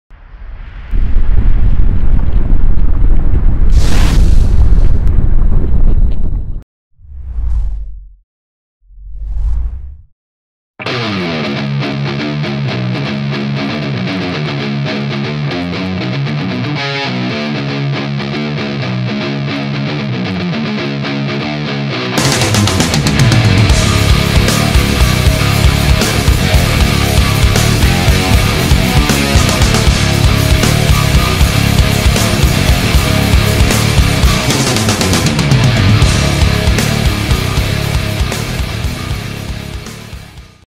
LNF - Workshop con Alexey Bakhirev
Workshop con Alexey Bakhirev sui 3 big alla Urbe Academy di Roma.
A breve verranno organizzati nuovi eventi con personaggi di alto livello.